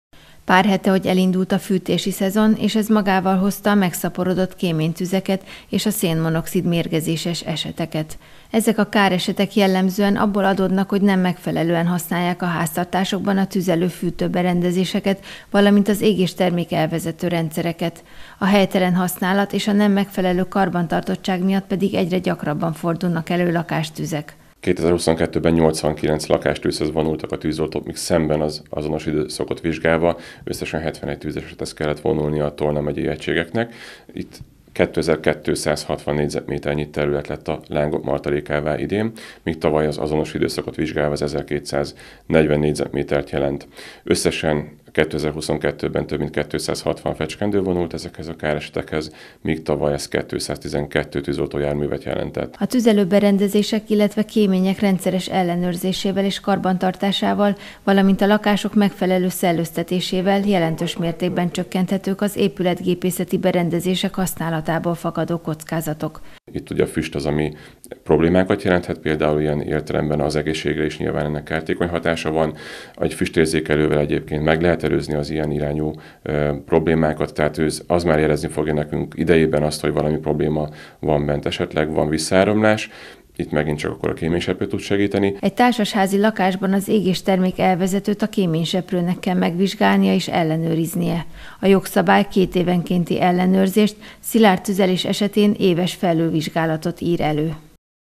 Egyre gyakoribbak a kéménytüzekből fakadó lakástüzek
Míg 2021-ben 71 lakástűzhöz riasztották a tűzoltókat, addig ez a szám 2022-re már 89- re nőtt. Közel 2300 négyzetméternyi terület vált a lángok martalékává a nem megfelelően használt tüzelőberendezések, illetve a nem karbantartott kémények miatt.